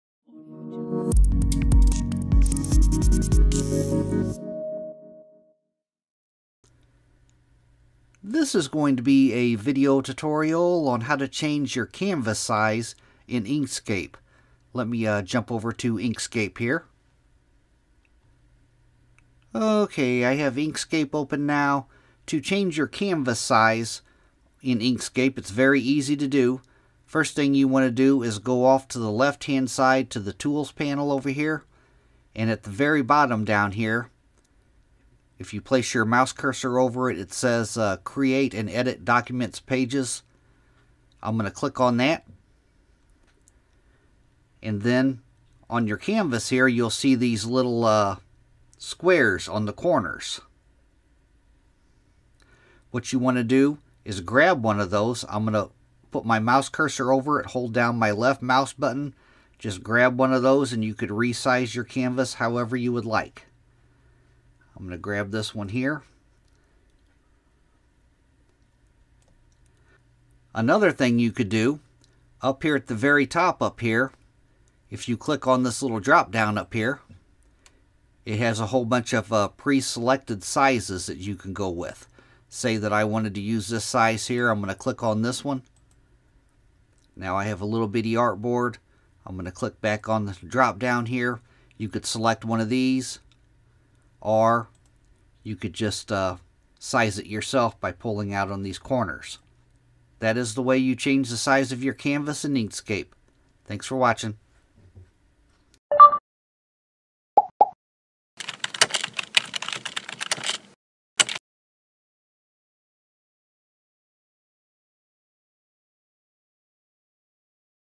Inkscape: How To Change The Canvas Size In Inkscape
This is a video tutorial on how to change the canvas dimensions in Inkscape. I will be using a desktop PC for this tutorial.

Simple Steps
1. Open your project in Inkscape
2. Select the "Create & Edit Document Pages" icon from the tools panel.
3. Hold down the left mouse button over one of the corner anchor points and drag to modify the size of the artboard.

Chapters
0:00 How To Resize The Canvas In Inkscape
0:17 Open Your Project In Inkscape
0:30 Select The Create And Edit Document Pages Icon In Inkscape
0:45 Grab One Of The Anchor Points On The Canvas To Resize
1:12 Another Way To Resize The Canvas In Inkscape Using Pres elected